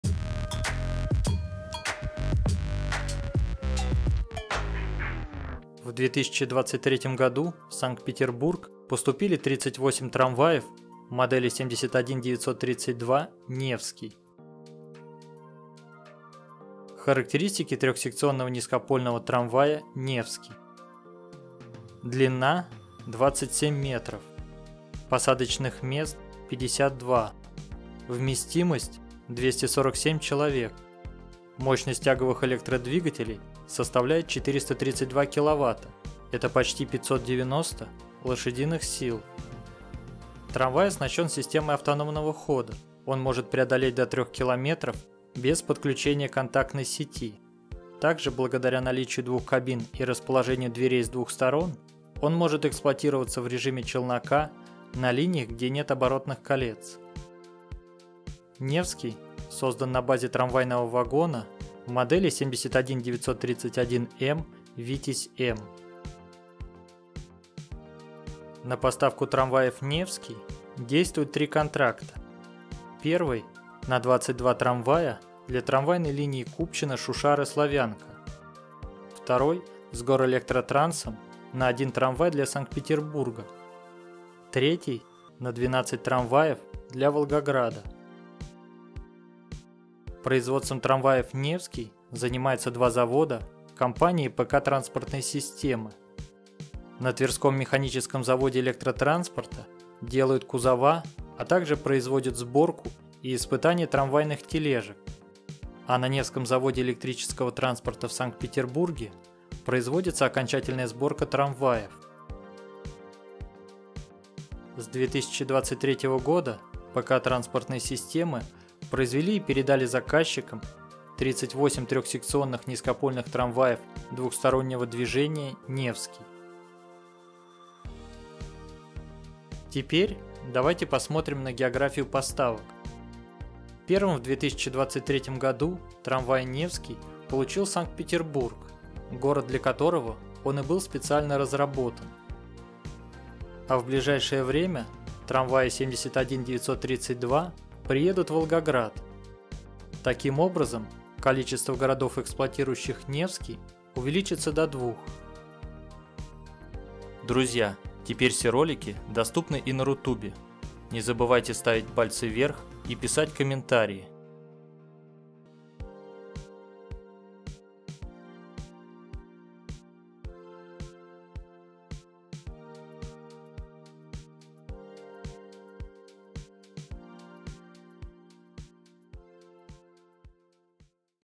Трамвай 71-932 «Невский» | Развитие городского электротранспорта 2024
Трамвай 71-932 «Невский» - это российский полностью низкопольный трёхсекционный трамвай двустороннего движения компании "ПК Транспортные Системы". "Невский" создан на базе трамвайного вагона 71-931М «Витязь-М». В его основе - уникальная низкопольная, поворотная, эластичная тележка собственной разработки и производства. Наличие двух кабин и расположение дверей по обе стороны бортов позволяет эксплуатировать трамвай «в режиме челнока».

В этом видео:
00:00 начало
00:16 характеристики
01:08 контракты
01:30 заводы
01:54 динамика поставок
02:09 география поставок
02:37 это интересно

Ссылки
Характеристики: